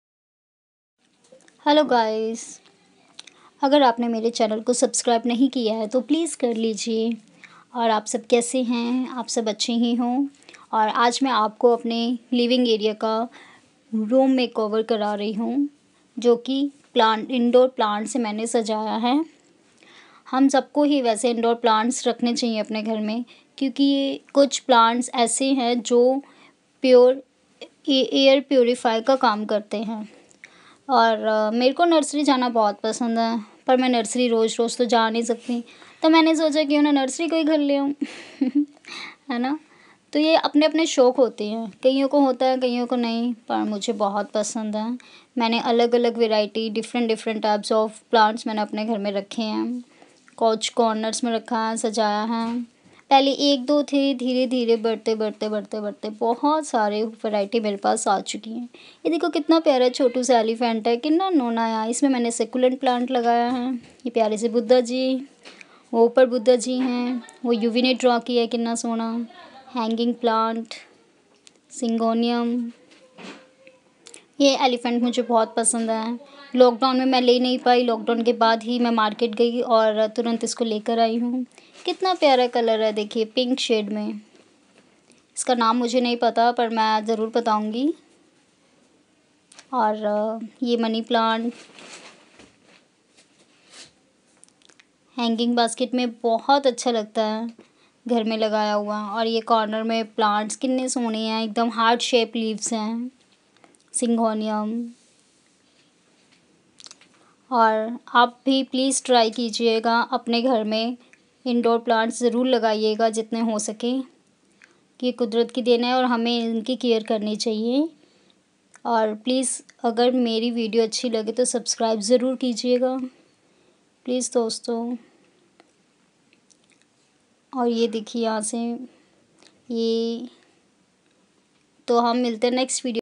My living area makeover with indoor plants
Hi 👋🏻 guys mene apne living area ko indoor plants se sajaya h mujhe umeed h aap b apne ghar me indoor plants lekar aayenge ☺️💕🌷☘️🌹🌸🌺🍀